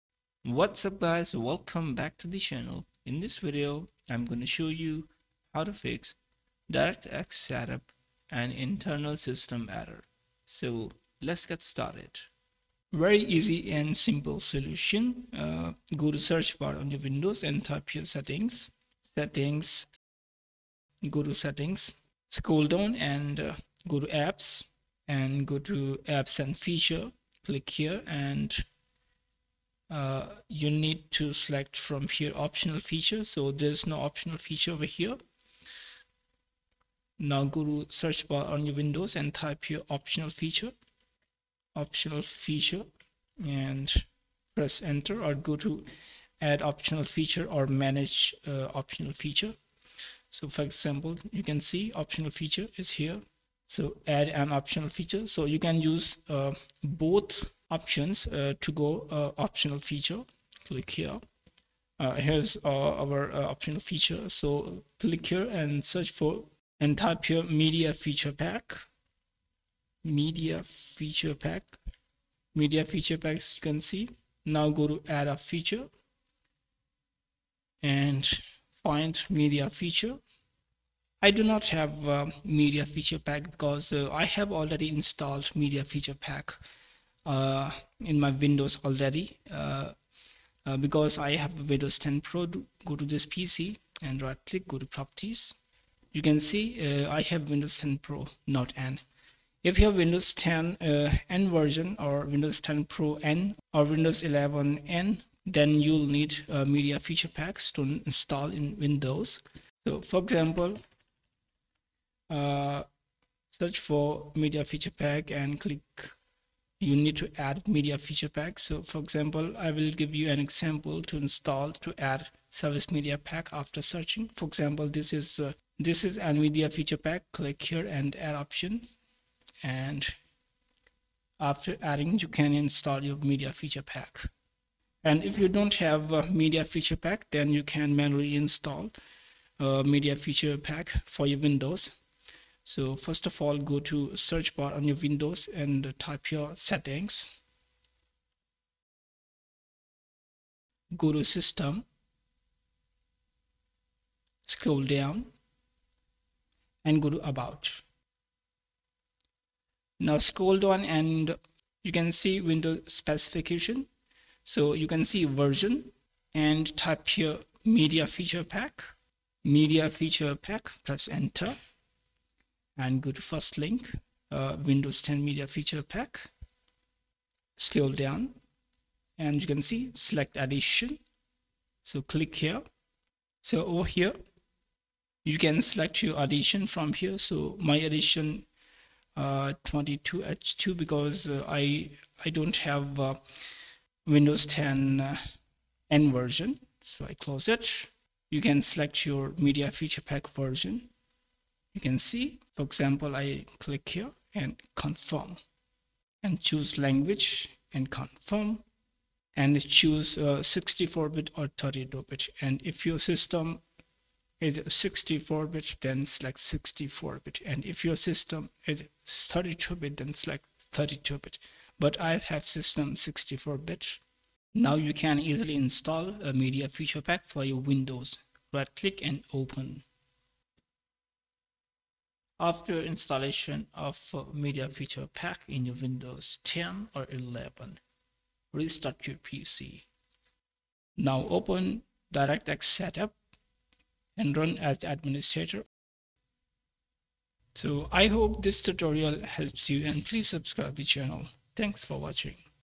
How to fix DirectX Setup Installer An internal System error Occurred
Hello,
In this video i have shown how to Fix DirectX Setup Installer An internal System error Occurred.